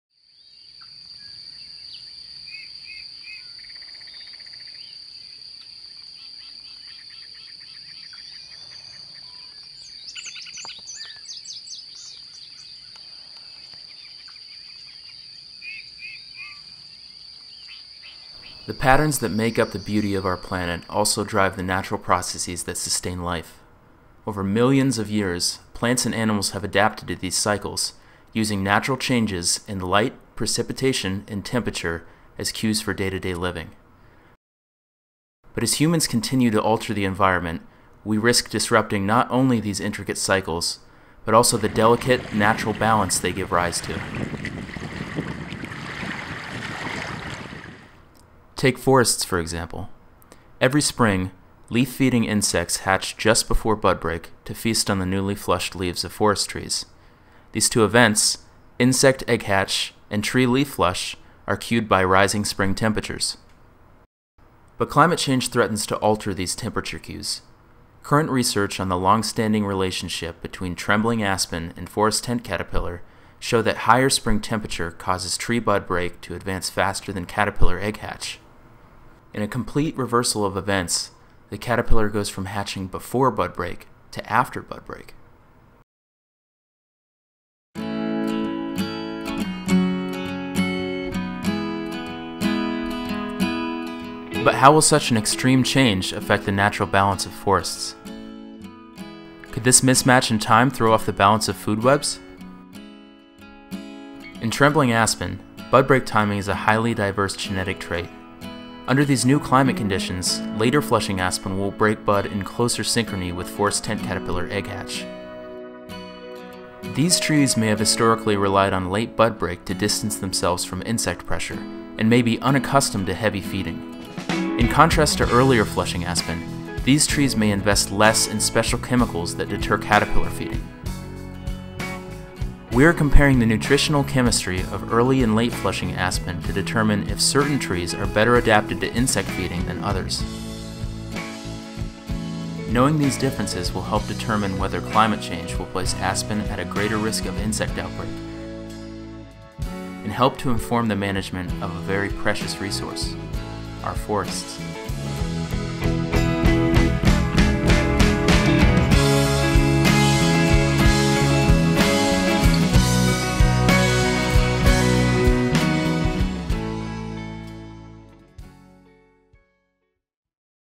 (A)synchrony - An Untimely Problem
How will climate change affect the seasonal timing of important tree-insect interactions? Will forests be subject to more insect pests? Join Michael as he embarks on his research exploring these interactions. This video was produced by University of Wisconsin Entomology students Michael Falk and Jeremy Hemberger.